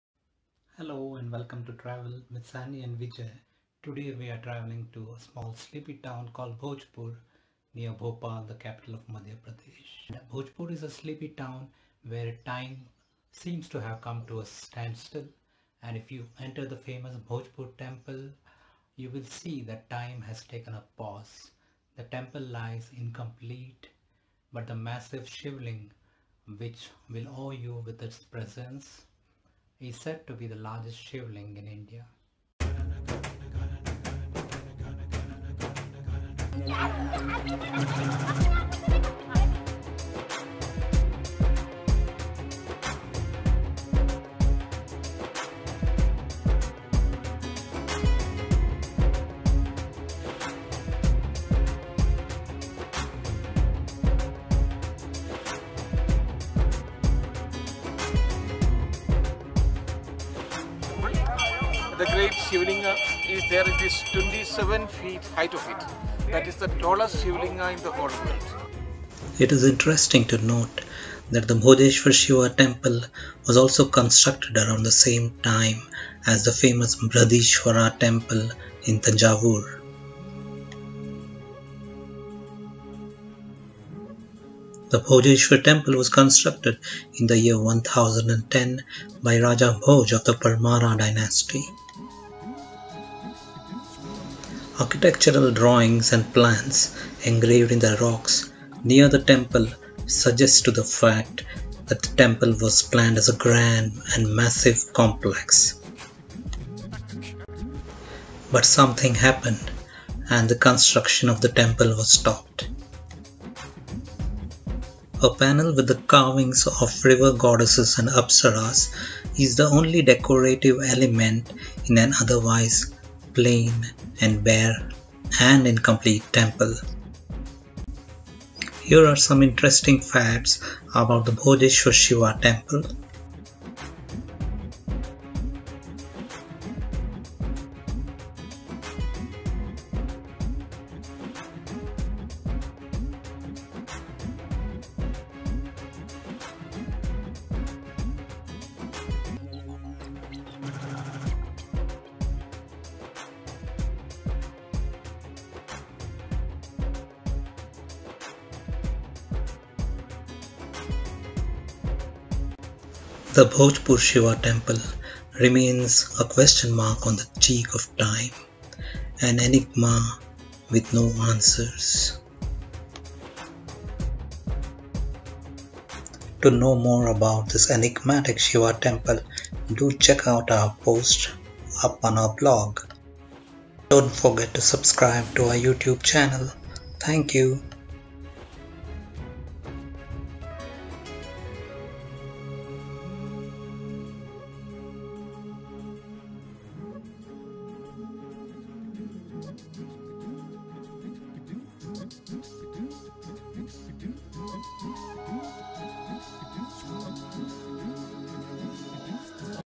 Bhojpur Shiva Temple - Mystery of Unfinished Temple of Madhya Pradesh - Largest Shiv Ling
The Bhojpur temple near Bhopal in Madhya Pradesh is an enigmatic and incomplete temple of Madhya Pradesh. It has one of the largest Shivlings in India and was built by Raja Bhoj.

00:00:32 End of Intro

Read our blog post on the Bhojeshwar Temple to plan your trip